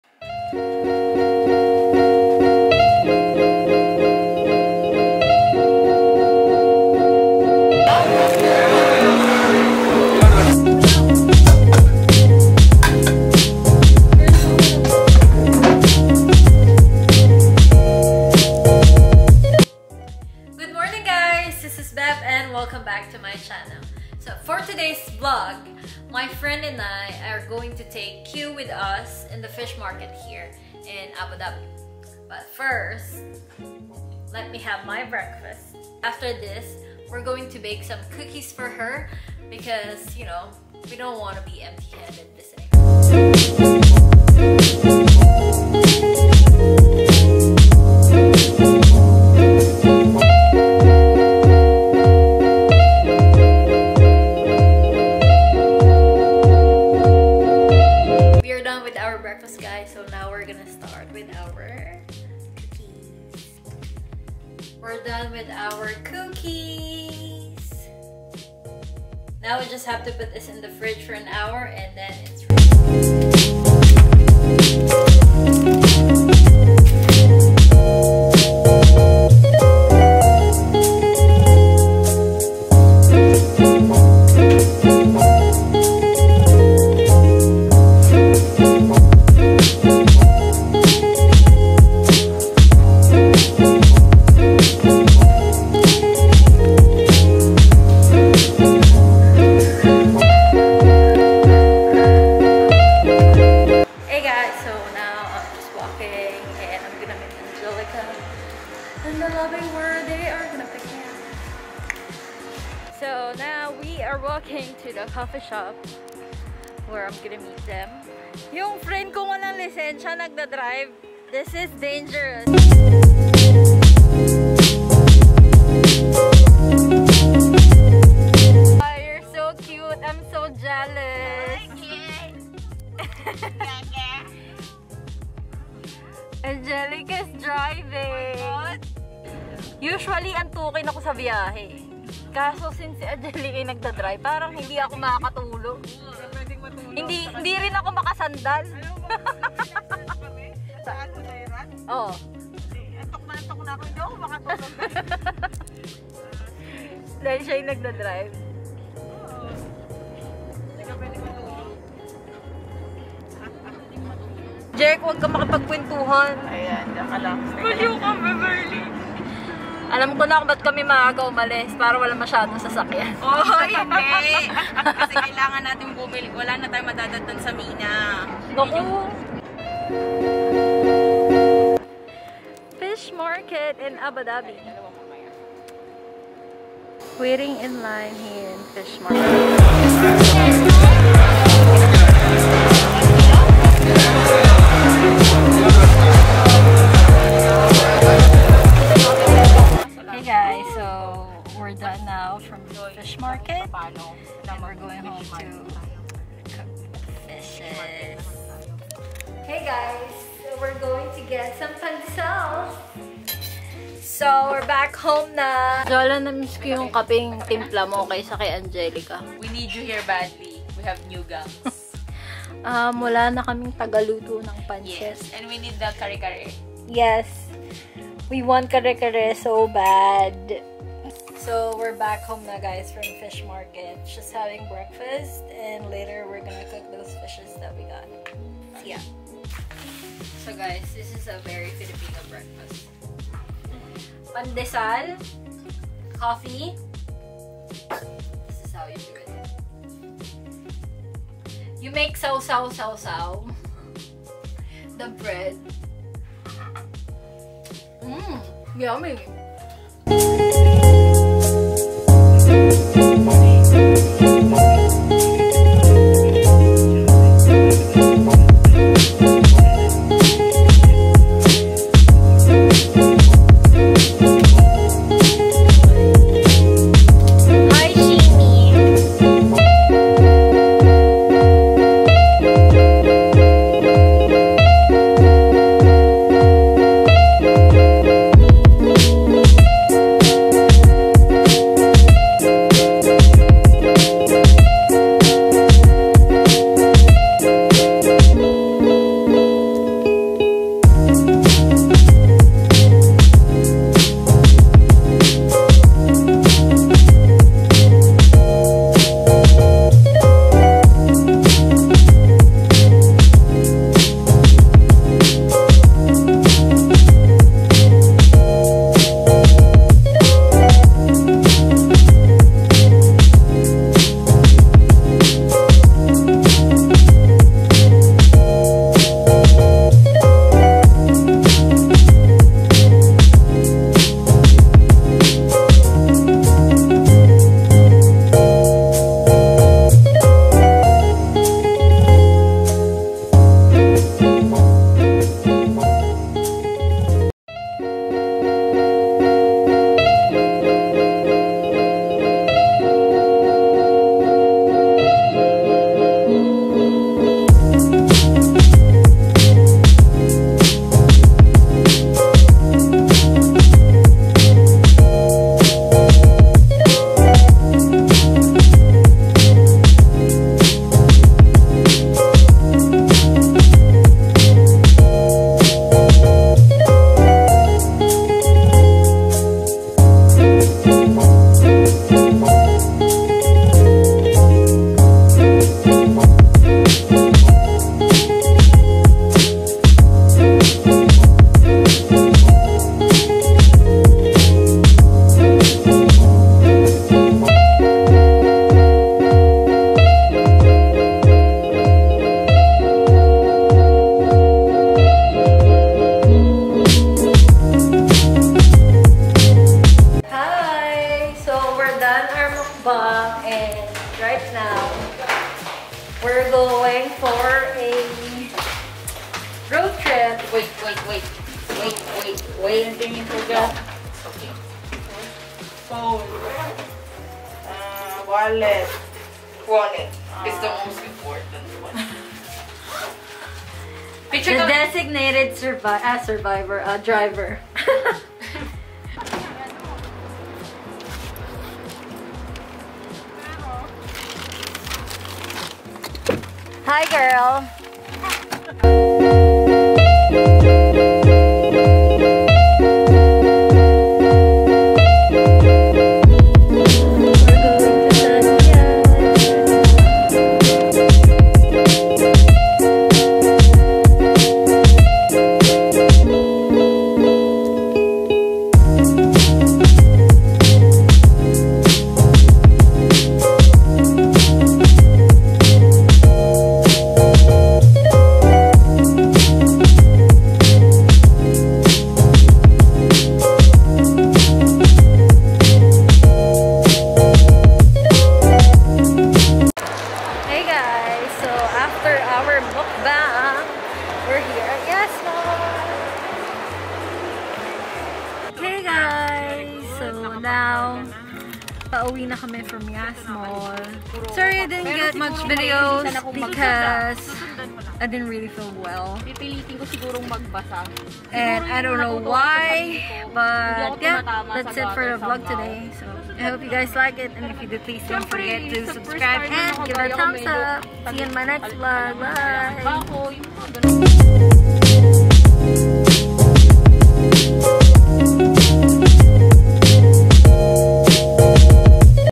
Trip to Abu Dhabi fish market + baking nutella cookies + Mukbang
Hello Guys!!!

Meet my friends!!!

Love lots,
bevziee